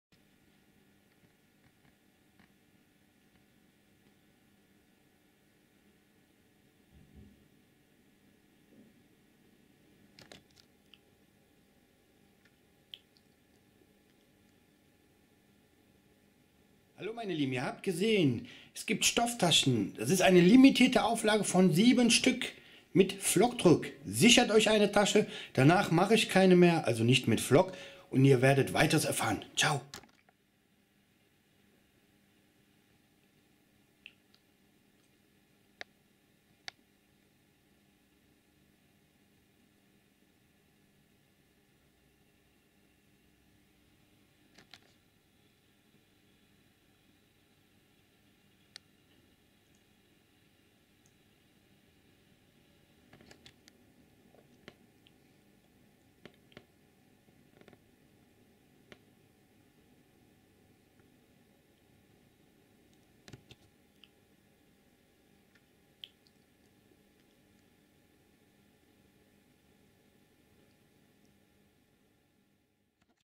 😍 [Limitierte Auflage] 😍
Nun ist es soweit Markus hat seine eigenen Stofftaschen fertig gestellt.

Einmalig und nur in 🔴 Limitierter Auflage 🔴  von 7 Stück erhältlich ! ! !

Greifft zu

Wenn Ihr Fragen, Vorschläge, Ideen, Anregungen habt schreibt Sie
gern in die Komis.

▶️ Mein Equipment: ◀️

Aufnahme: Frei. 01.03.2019
Text: MB
Kamera: MB